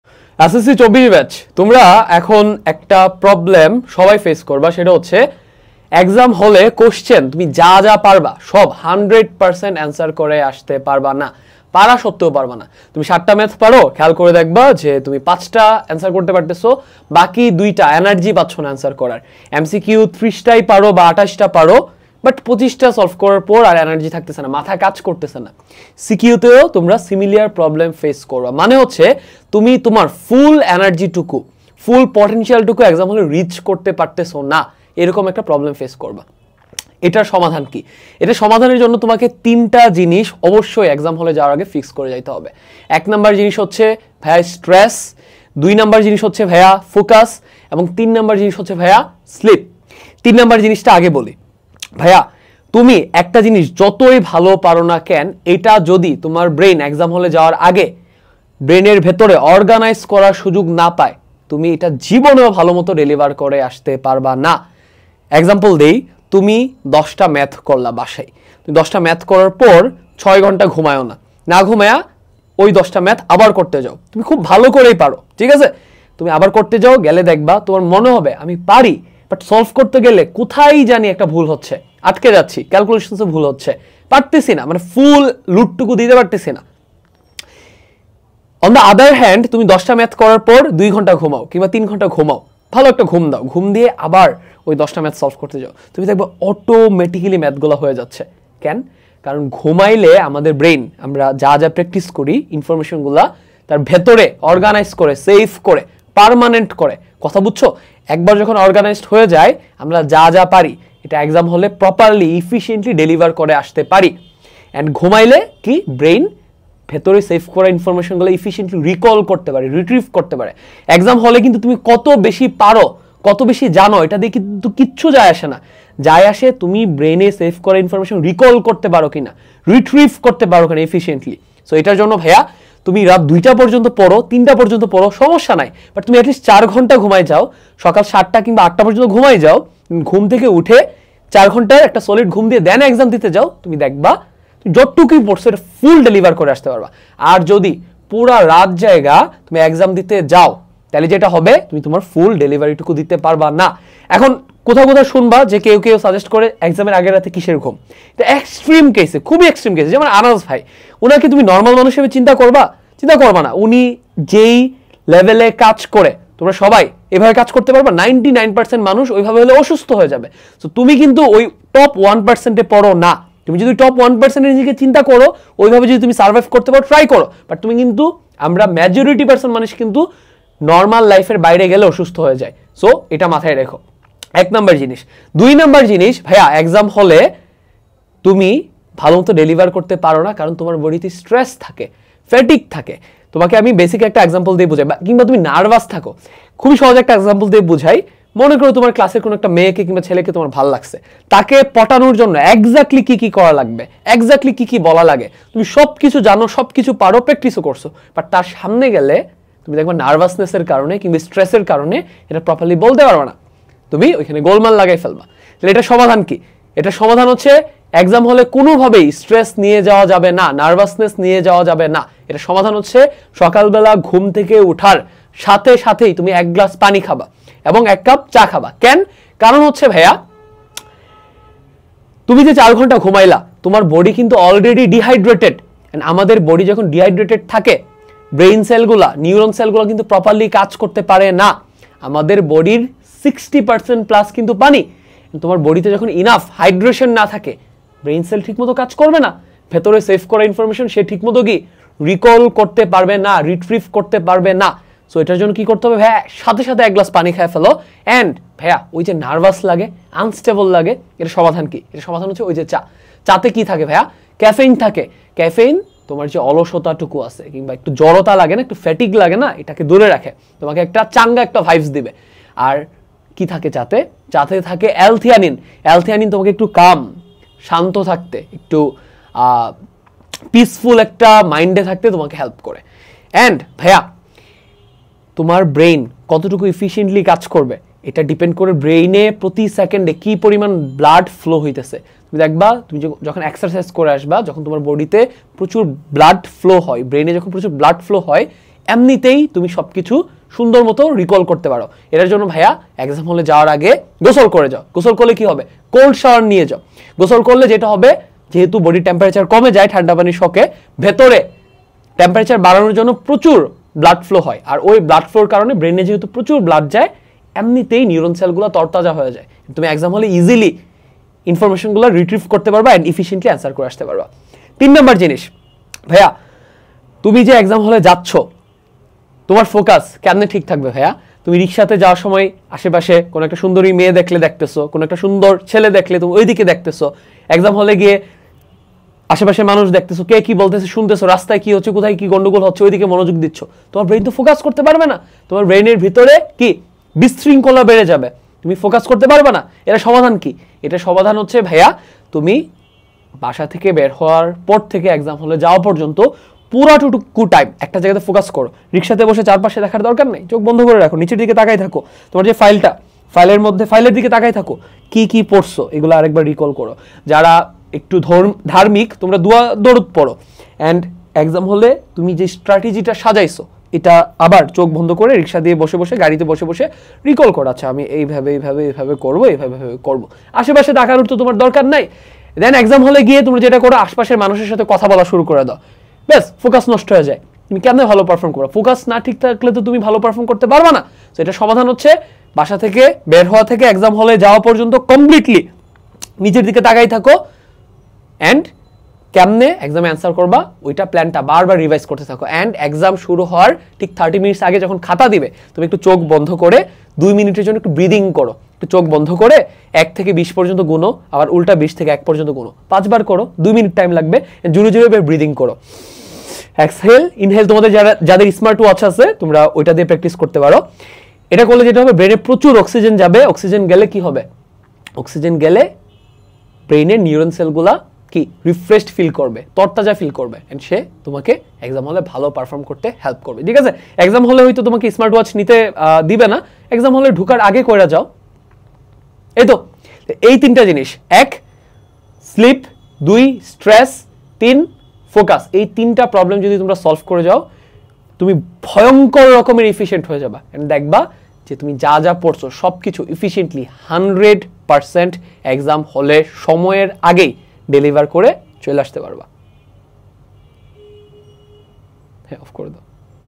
SSC 24 & 25 | এই ৩ টা সহজ HACK অ্যাপ্লাই করে FULL ENERGY নিয়ে পরীক্ষা দিতে পারবা
*হাতে সময় কম থাকলে 1.5X স্পিডে দেখো ভিডিওটা*
*দেখা হচ্ছে বিজয়ে* 💪

Instructor
Sayeed Islam Hridoy
Studying CSE at MEC, DU Technology Unit